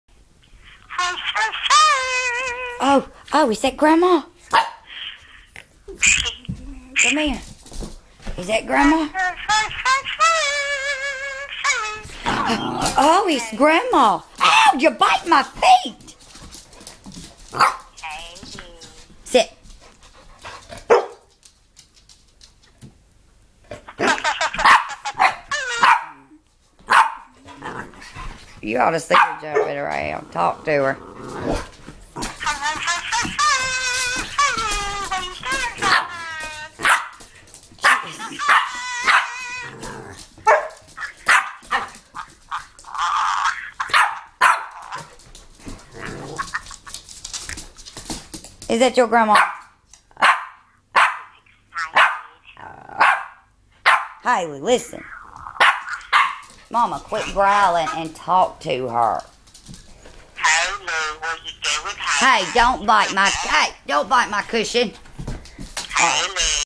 Haley talking to grandma
Haley cannot go a day without speaking to my mom on the phone. She will actually CRY to talk to her